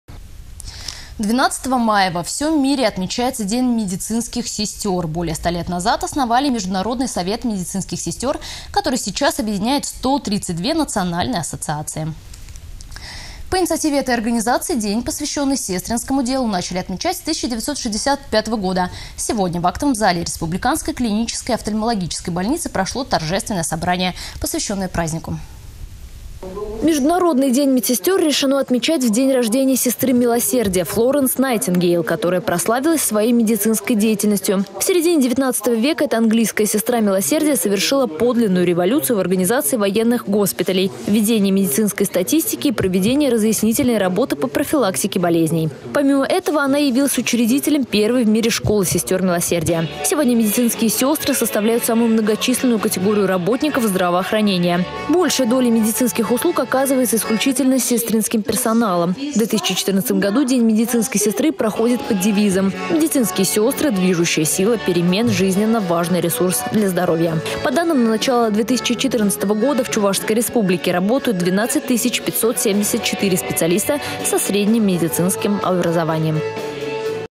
12 мая - День медицинских сестер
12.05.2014
12 мая во всем мире отмечается День медицинских сестер. Более 100 лет назад был основан Международный совет медицинских сестер, объединяющий на сегодняшний день 132 национальные ассоциации. По инициативе этой организации день, посвященный сестринскому делу, начали отмечать с 1965 года. Сегодня в актовом зале Республиканской клинической офтальмологической больницы прошло торжественное мероприятие, посвященное празднику.